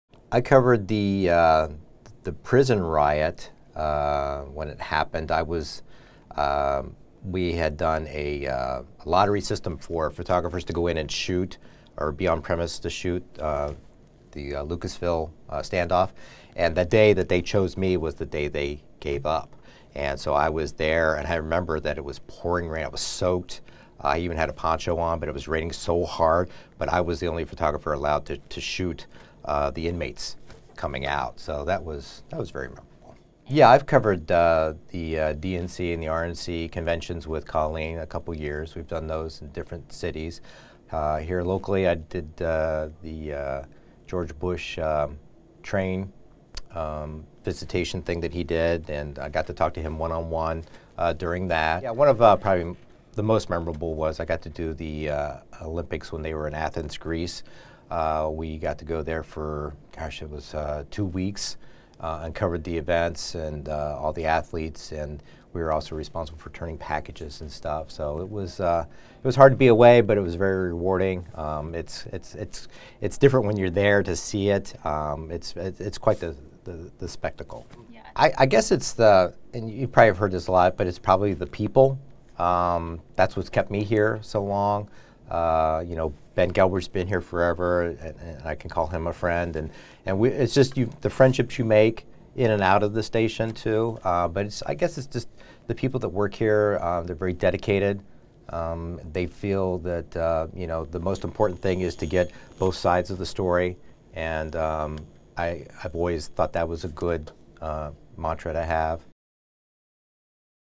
Steve Wainfor Long-time NBC4 employees reflect on WCMH 70th anniversary
Steve Wainfor has been with WCMH for nearly 33 years. Steve has been all over the country and as far away as Athens, Greece,  covering events for NBC4 as a special projects producer and photographer.